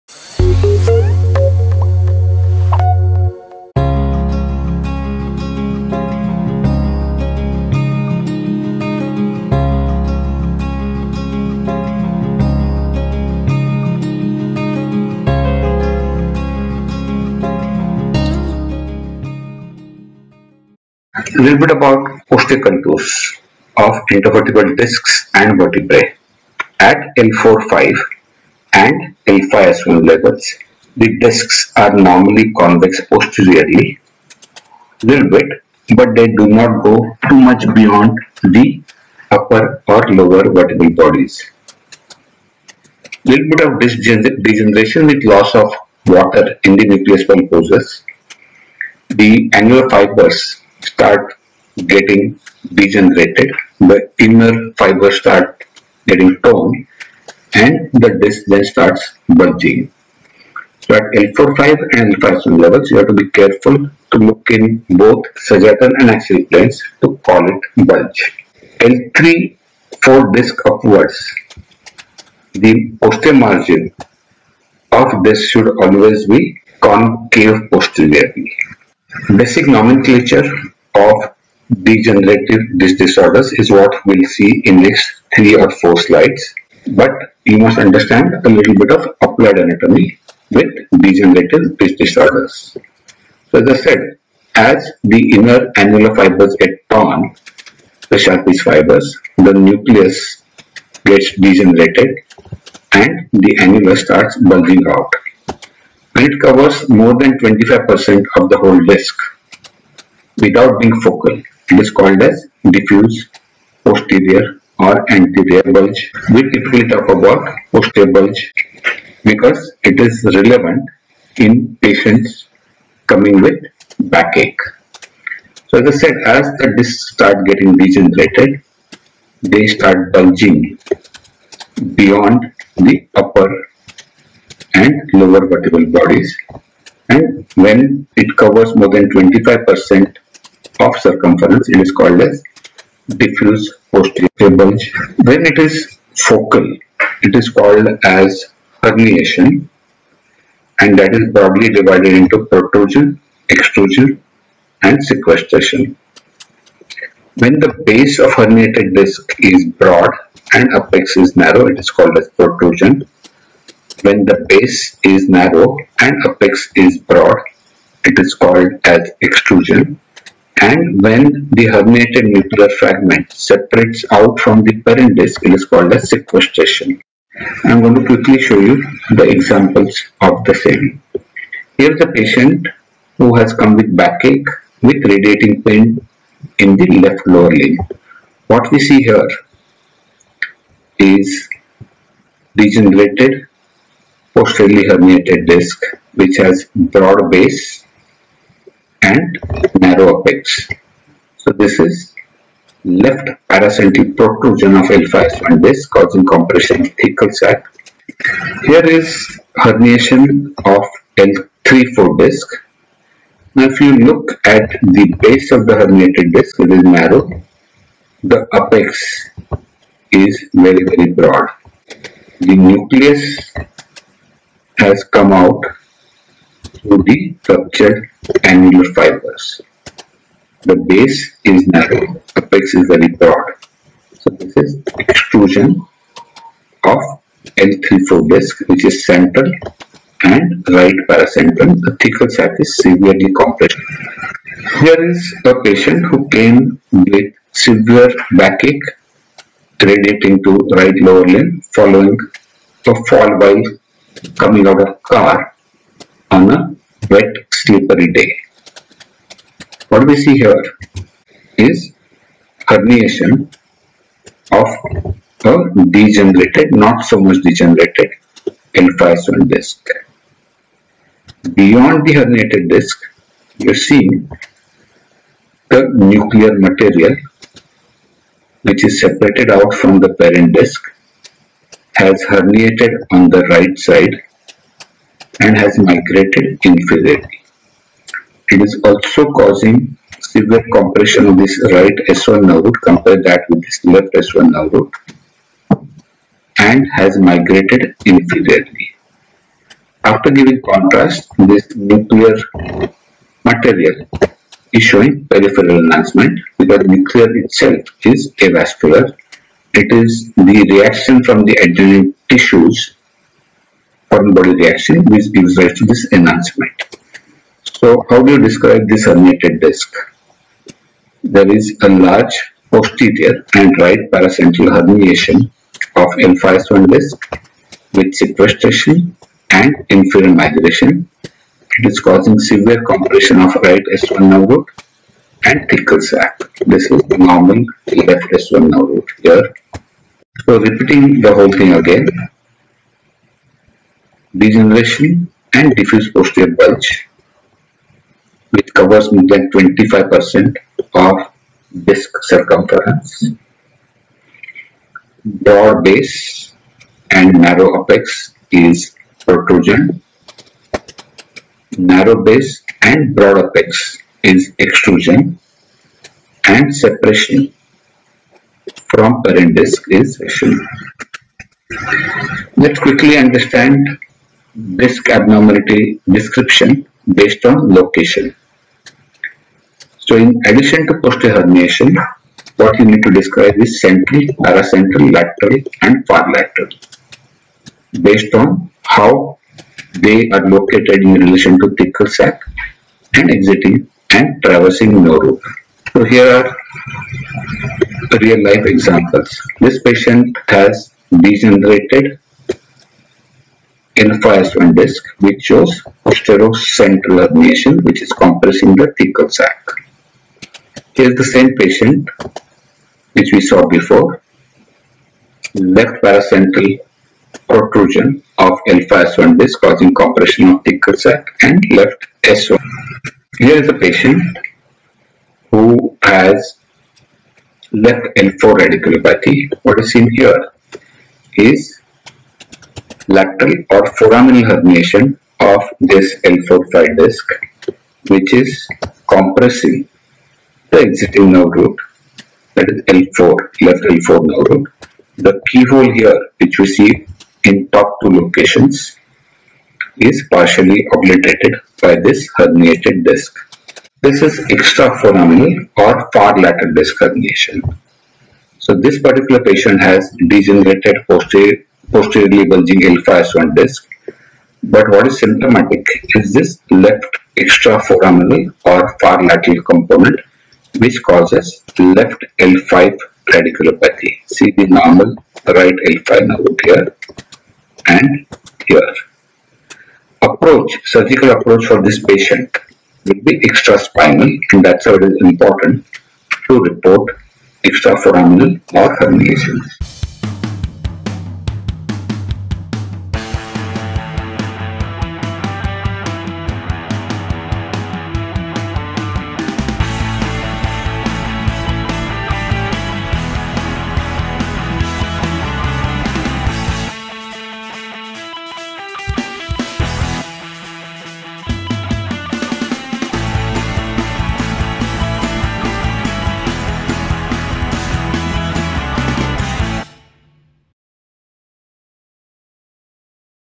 BASIC CONCEPTS OF DEGENERATED INTERVERTEBRAL DISC ON MRI | Use 🎧 for this video | DR DEEPAK PATKAR
Headphone 🎧